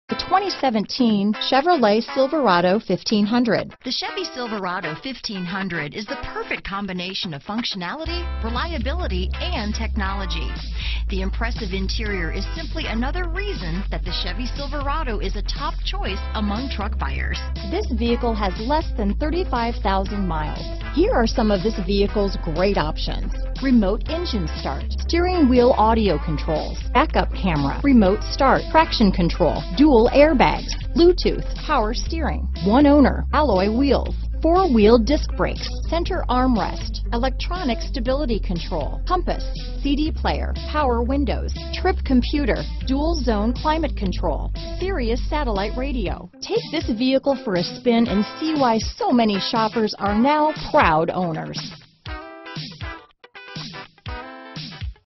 2017 Chevrolet Silverado 1500 Alliance, Akron, Canton, Youngstown, Cleveland, OH N160
Silver Ice Metallic U 2017 Chevrolet Silverado 1500 available in Alliance, Ohio at Lavery Automotive. Servicing the Akron, Canton, Youngstown, Cleveland, OH area.

Lavery Automotive
1096 W State St
Alliance OH 44601